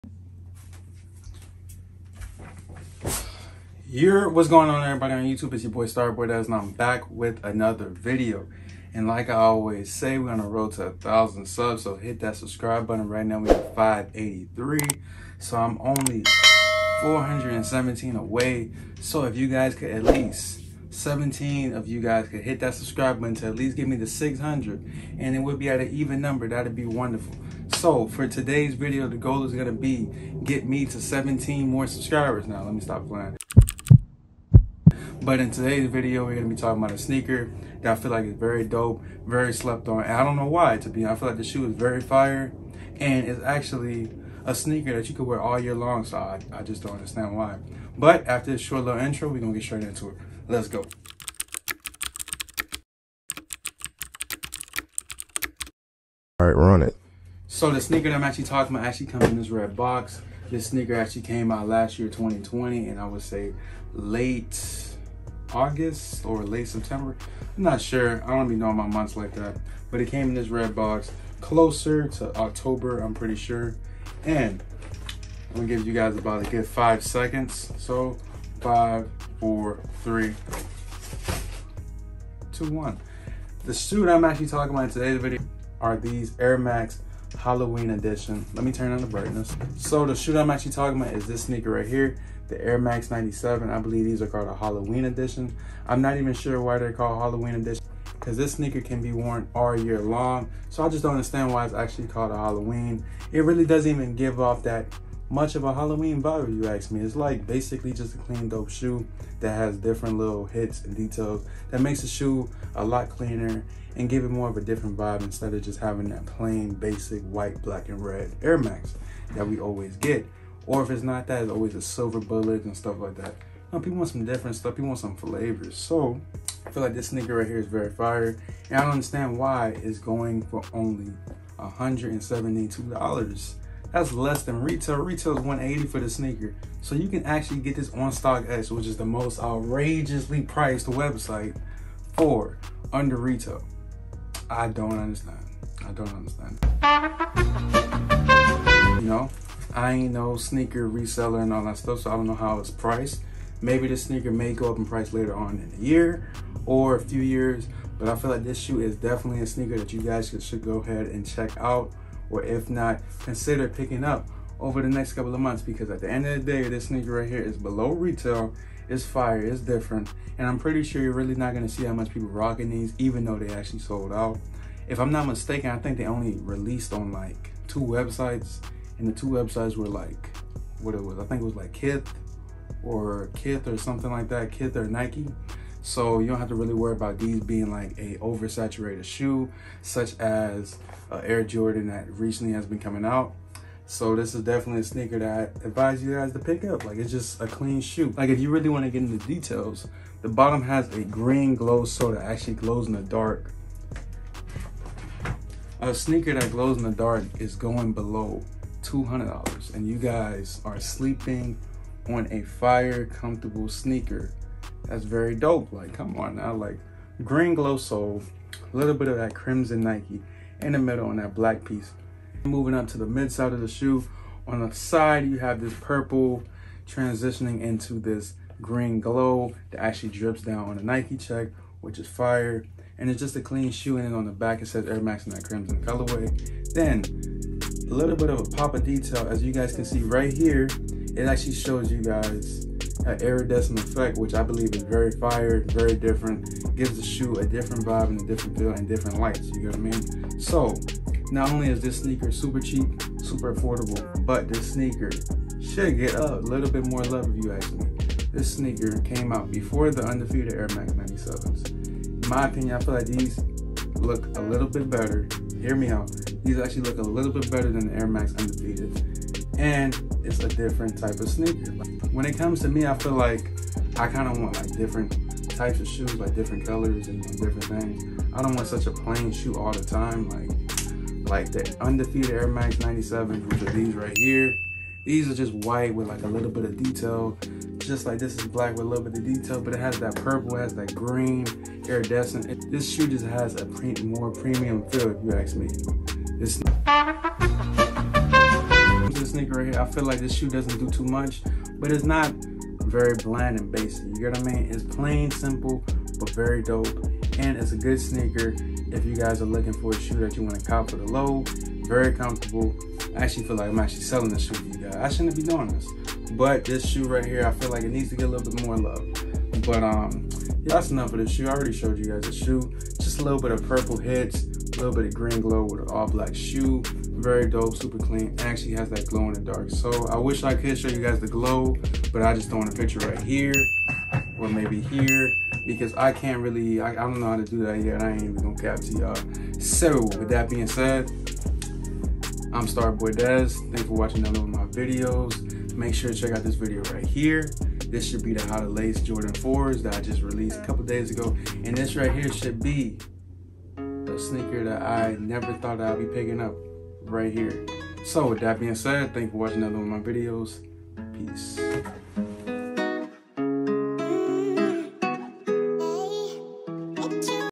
IS THIS THE BEST AIR MAX SNEAKER | HALLOWEEN AIR MAX 97 REVIEW
is this the best air max sneaker | Halloween air max 97 review is a question i need you guys to answer down below. Do you guys believe this sneaker is the best air max  or is it trash. This halloween air max 97 is very dope and i belive you guys will definetly enjoy this sneaker. The Halloween air max 97 is a sneaker that is very dope and comfortable.  So lmk Is this the best air max sneaker?